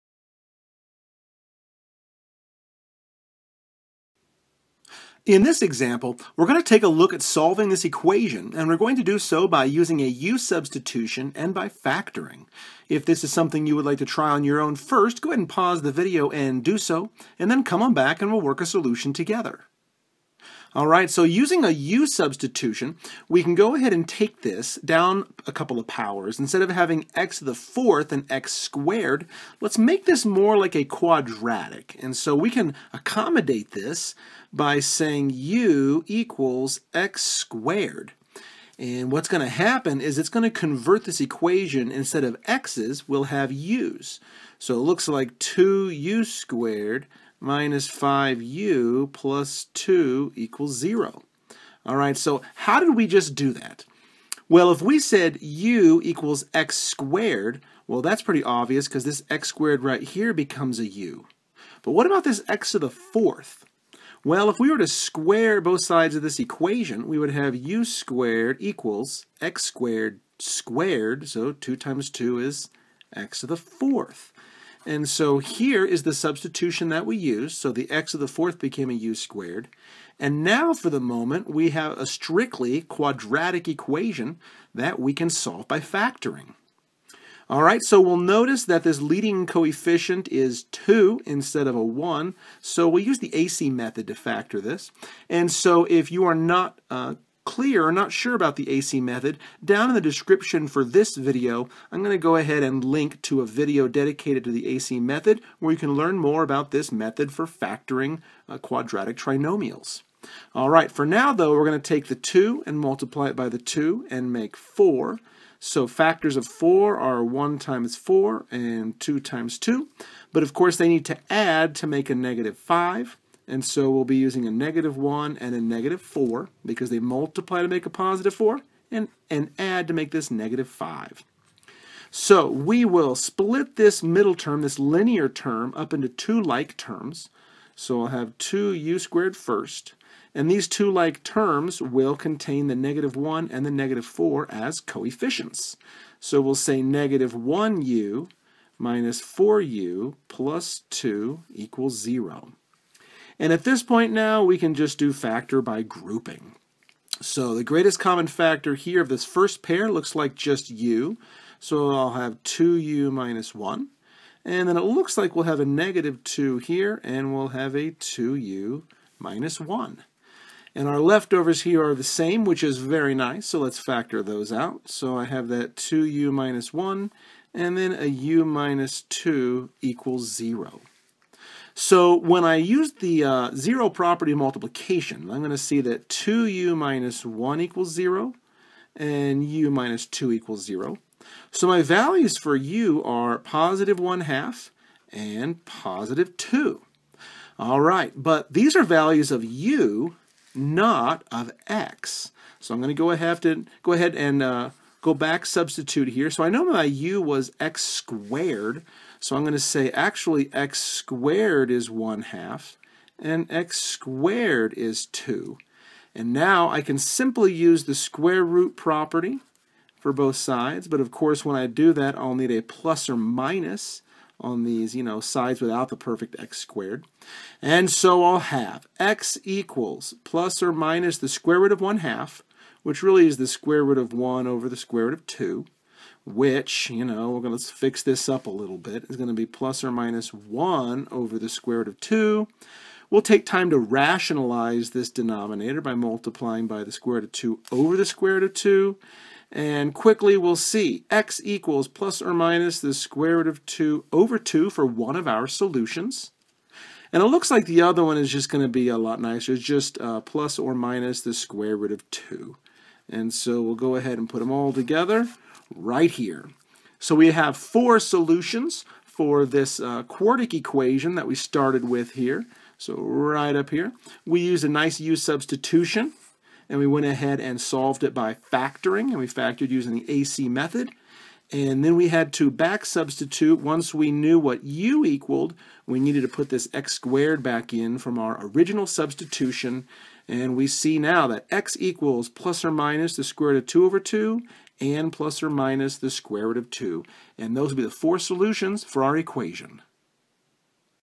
Solve by Factoring Using a u-Substitution (Example)
This video walks through an example of how we can use a u-substitution to create a quadratic equation to solve this higher-degree quartic equation.

The quadratic trinomial is factored using the AC Method. More information on the AC Method can be found by watching this video: